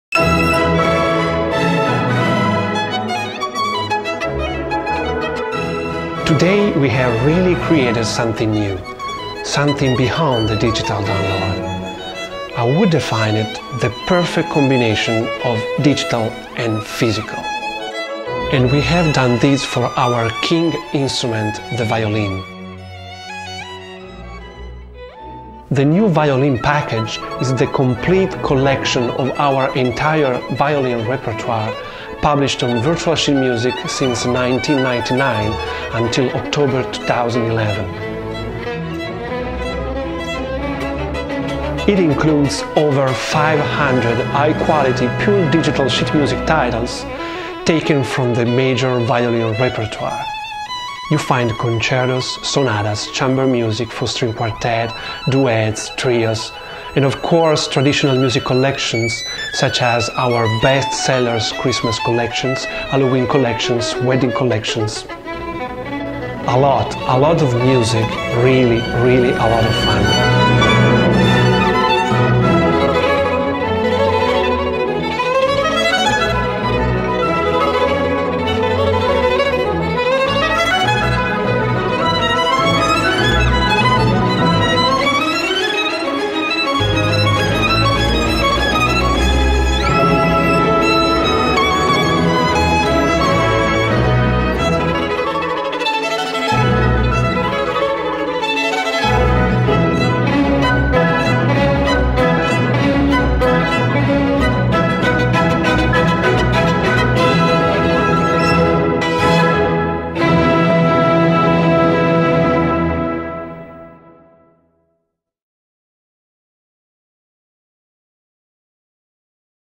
Violin Sheet Music Package by VSM - Best violin gift
Enjoy the video presentation of the new Virtual Sheet Music Violin Package.

The new Violin Package may be the perfect gift for a violinist, a violin teacher or simply any violin lover.

Questions and comments are very welcome!